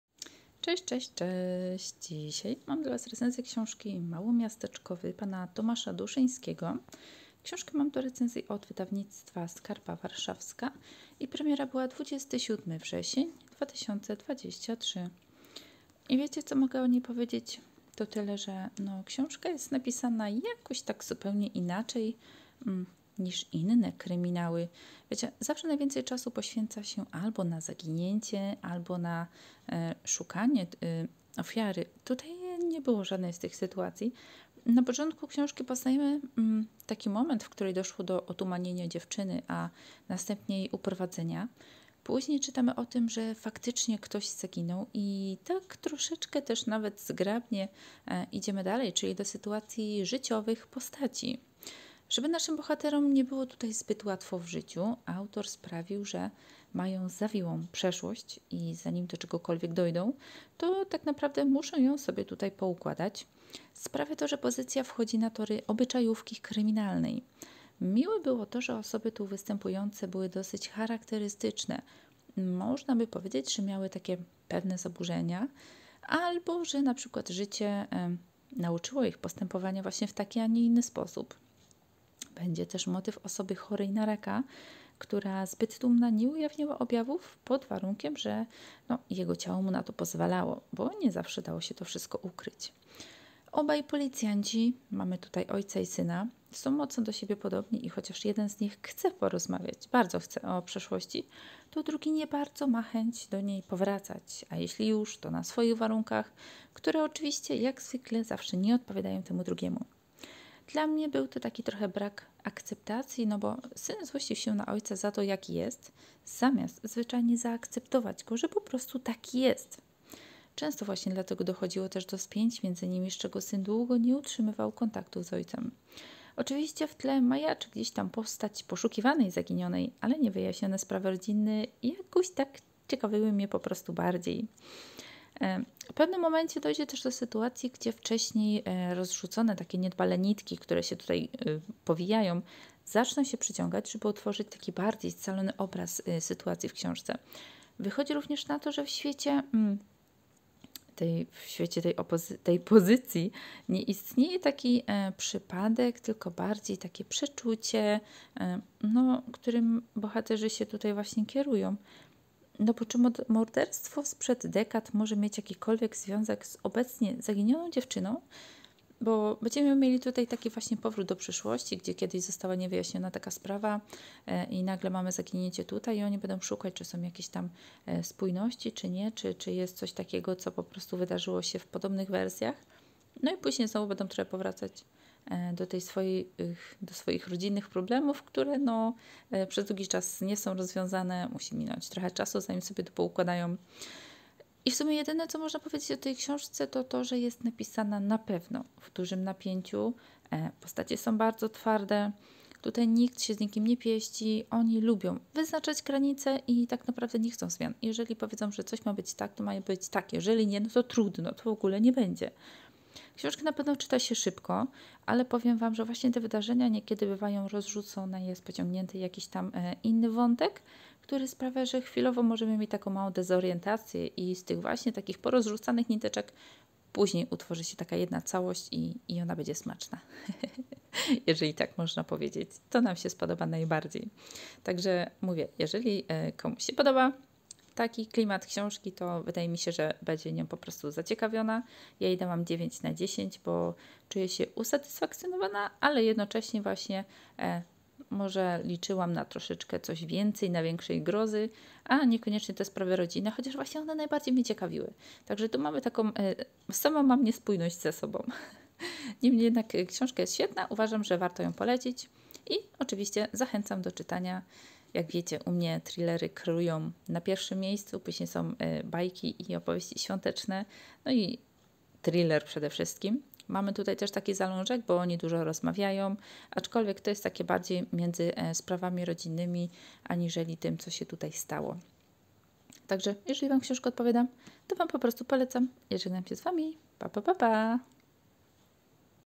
,, Małomiasteczkowy" Tomasz Duszyński
Premiera 27 września 2023r.
Dziękuję Wydawnictwu Skarpa Warszawska za książkę do recenzji🥰